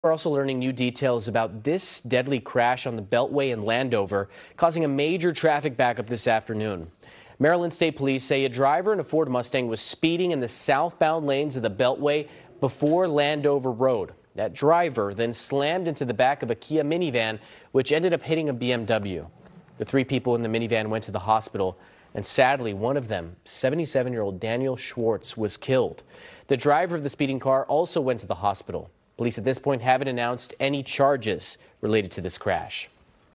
Man dies after speeding driver rear-ends van on Beltway in Landover: police | NBC4 Washington
A crash involving three cars left one man dead and closed several lanes of Interstate 495 in Landover, Maryland, on Sunday.